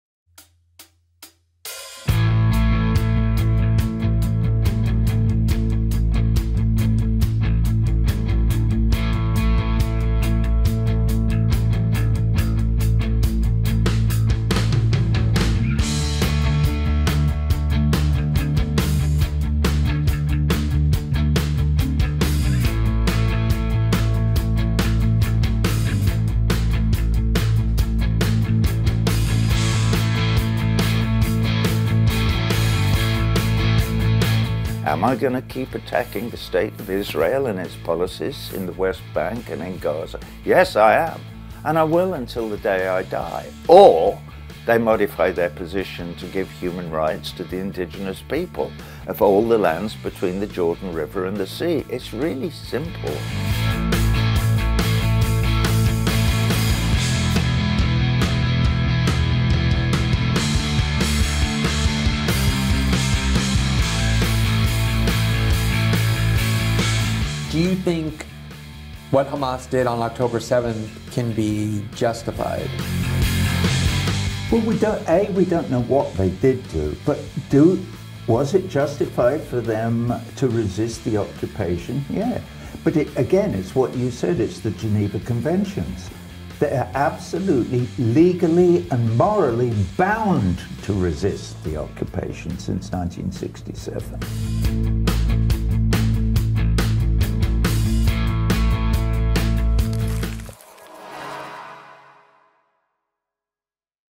مغن بريطاني يعبر عن تضامنه مع غزة بحفل موسيقي في أوروغواي
واضعا كوفية فلسطينية حول رقبته.. مغن بريطاني يعبر عن تضامنه مع غزة بحفل موسيقي في أوروغواي